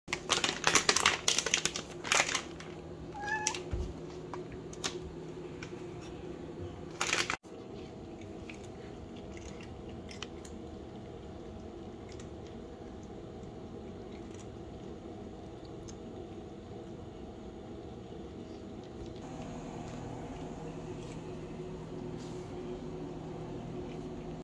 treat tax
Shisha always steals one from Miss Juicy or Pumpkin. No matter how many I give her, she just has to take one from them she loves them.